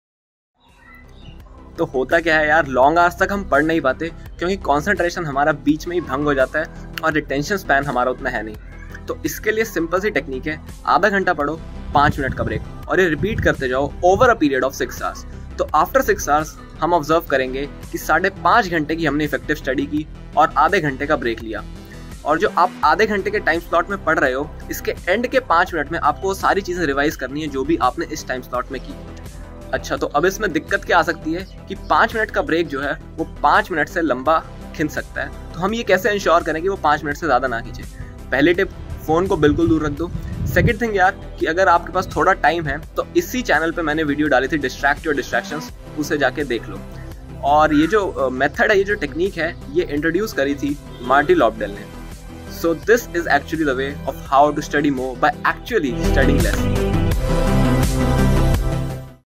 Study more by studying less | Sarthak Sharma
In this video I have discussed the technique to study smarter than studying longer.
Quick Overview:
• Study for 30 mins then take a 5 mins break.
• Repeat this cycle to over 6 hours.
• At the end of 6th hour we will notice we have done an effective and productive studying of 5.5 hour and the break consists of 0.5 hour.
• In order to increase the efficiency of this technique in the time slot of 30 mins keep the last 5 mins for revision.